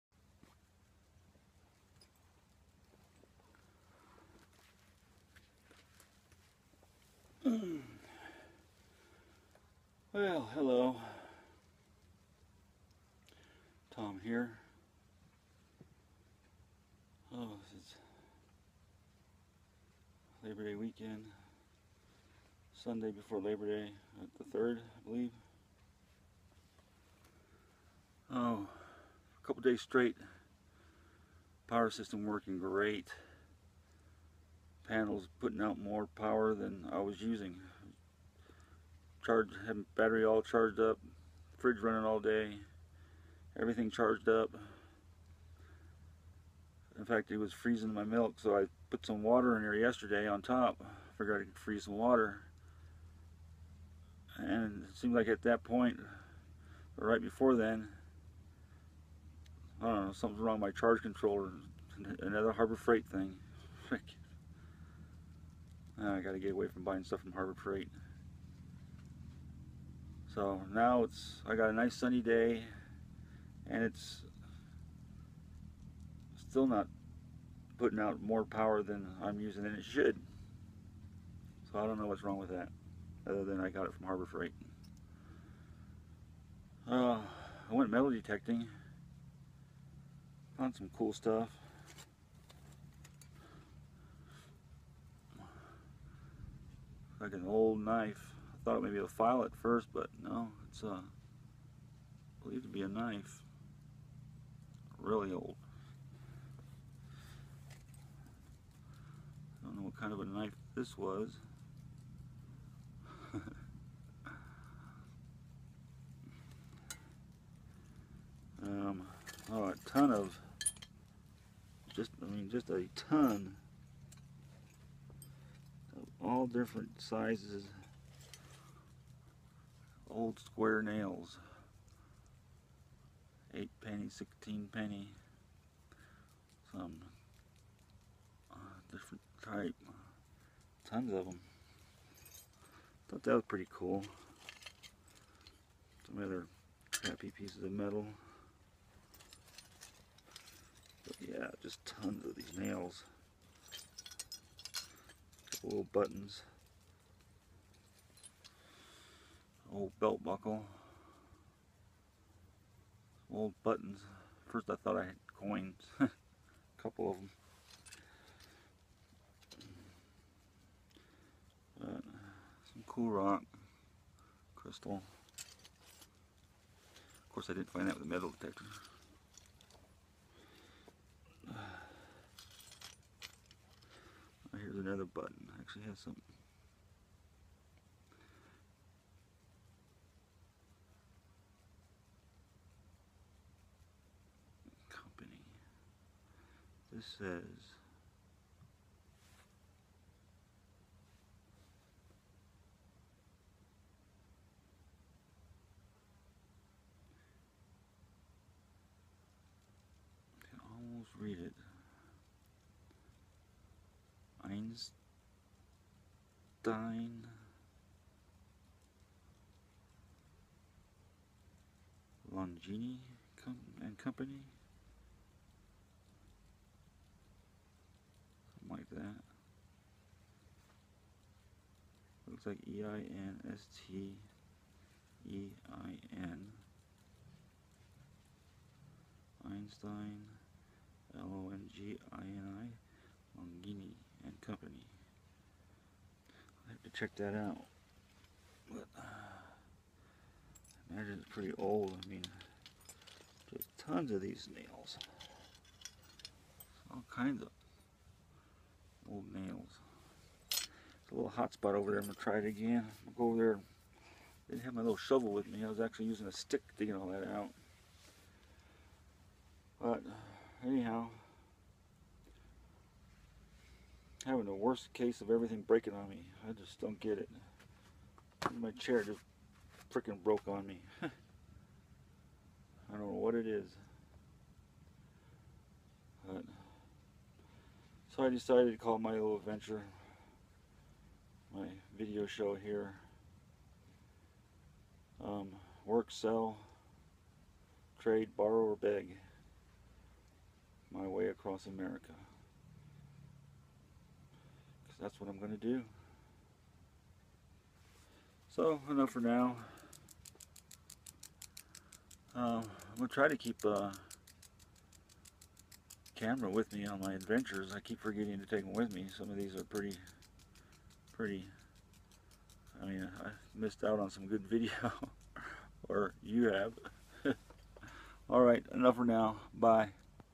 Adventures in camping with Tom/journey in camping by way of work/sell/trade/borrow or beg
Some commentary on my adventure and showing off my metal detector findings